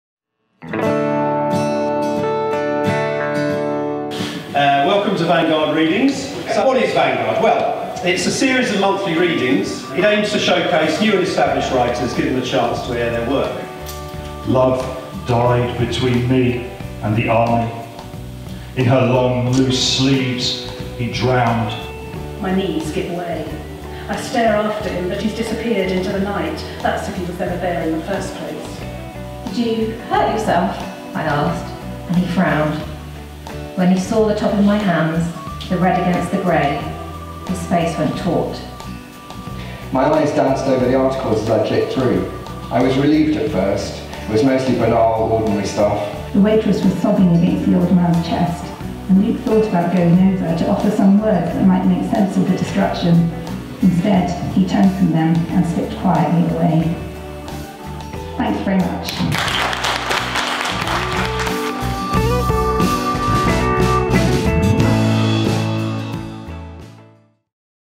Vanguard Readings Margate Bookie Nov 2019
Vanguard Readings is a series of short readings by new writers and established authors, taking place every few months in London. But everyone deserves a holiday by the sea, so they are now a key part of Margate Bookie. Come along and hear tomorrow’s authors today.

Renée Knight was a documentary-maker before turning to writing. She is a graduate of the Faber Academy ‘Writing a Novel’ course, where she began her first novel Disclaimer. Disclaimer went on to become a Sunday Times number one bestseller.Her second novel, The Secretary, was published this year.

Phoebe Locke is the pseudonym of full-time writer Nicci Cloke. She previously worked at the Faber Academy, and hosted London literary salon Speakeasy. She lives and writes in Cambridgeshire. Phoebe Locke is the author of two psychological thrillers, The Tall Man (2018) and The July Girls, which was published this summer.

Jeff Hilson has written four books of poetry: stretchers (Reality Street, 2006), Bird bird (Landfill, 2009), In The Assarts (Veer, 2010) and Latanoprost Variations (Boiler House Press, 2017). A fifth book, Organ Music, is due out soon from Crater Press. He also edited The Reality Street Book of Sonnets (Reality Street, 2008). He runs Xing the Line poetry reading series in London, and is a Reader in Creative Writing at the University of Roehampton.

Lesley Kara is an alumna of the Faber Academy ‘Writing a Novel’ course. Previously a lecturer and manager in Further Education, she now lives on the North Essex coast and writes full-time. Her debut novel The Rumour was a Sunday Times Top Ten hardback and paperback bestseller. Her second novel, Who Did You Tell?, is coming soon.

S J Watson’s first novel, Before I Go To Sleep, is a phenomenal international success. A bestseller around the world, it won The Crime Writers’ Association Award for Best Debut Novel and The Galaxy National Book Award for Crime Thriller of the Year. The film of the book, starring Nicole Kidman, Colin Firth and Mark Strong, and directed by Rowan Joffe, was released in September 2014. SJ Watson was born in the Midlands and now lives in London.

Helen Young is an author and digital editor. Breakfast in Bogotá, her second novel, published in August of this year with Unbound. Set in 1940s Colombia, the novel is about a broken architect trying to build something new. She will be reading from the first few chapters.